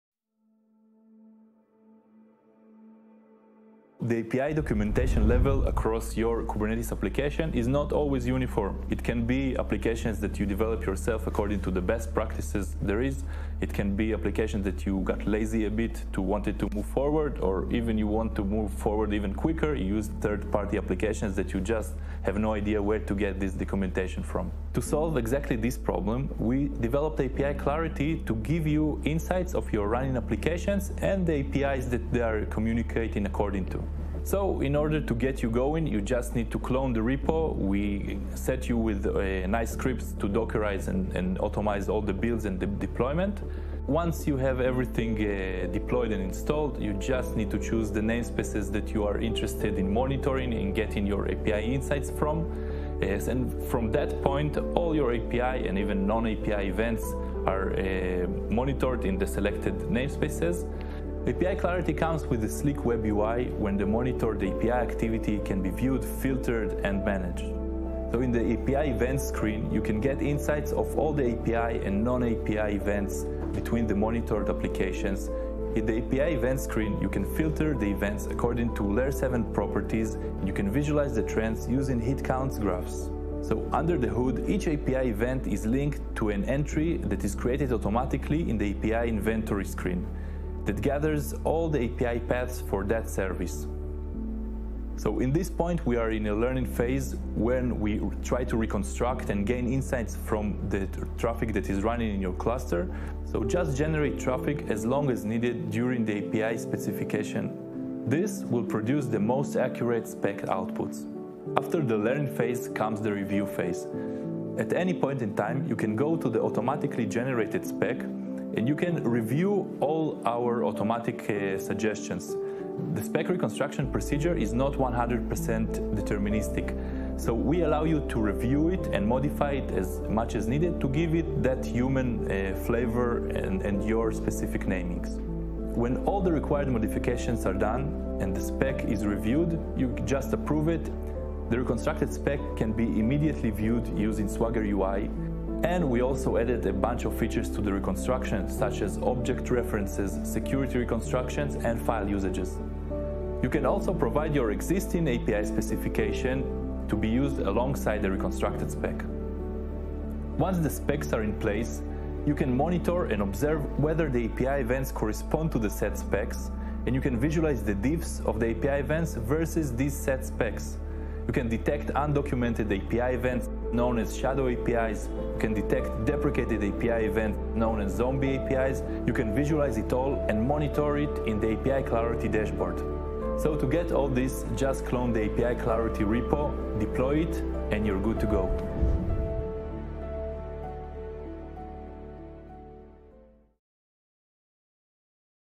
APIClarity Demo
The API documentation level across your Kubernetes application is not always uniform, it can be applications that you develop yourself according to the best practices, it can be applications that you got lazy because you wanted to move them forward, or even because you wanted to move forward even quicker using third party applications that you just have no idea where to get this recommendation from.
To solve exactly this problem, we developed API clarity to give you insights of your running applications and the API's that they are communicating with.